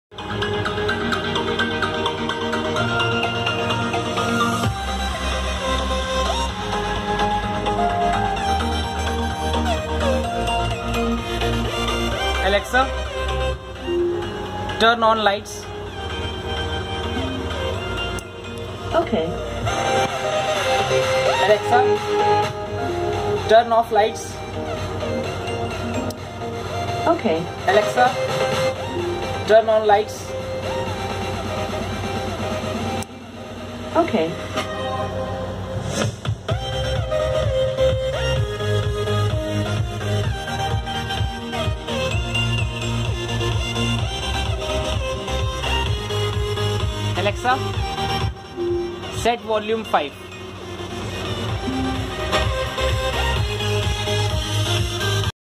Alexa power relay setup.. with raspberry pi 3 model b and power relay...
I havs used Amazon echo dot 2nd gen... Raspberry pi 3 model b, one power relay which are connected with gpio pin...